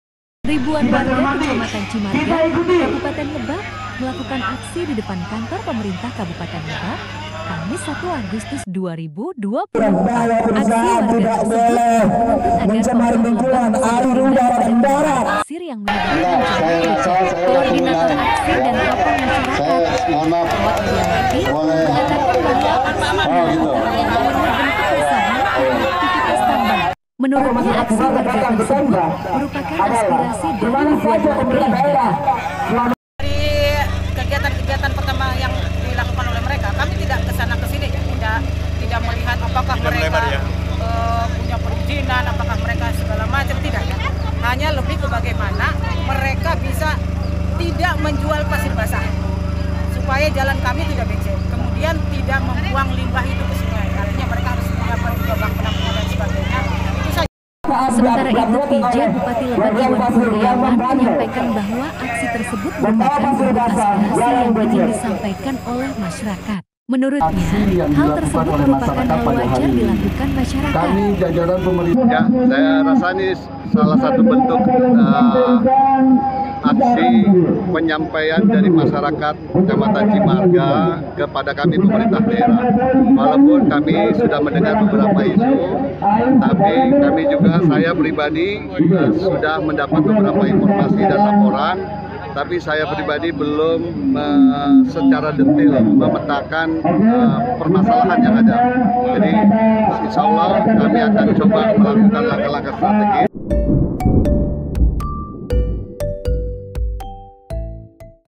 Minta Jalan Tidak Becek, Ribuan Warga Demo di Depan Kantor Pemkab Lebak | RADAR BANTEN
Ribuan warga Kecamatan Cimarga, Kabupaten Lebak, melakukan aksi di depan Kantor Pemerintah Kabupaten Lebak, Kamis 1 Agustus 2024. Aksi warga tersebut, menuntut agar Pemkab Lebak bisa bertindak kepada penambang pasir yang menyebabkan jalan becek.

Reporter : Nurandi
Video      : Nurandi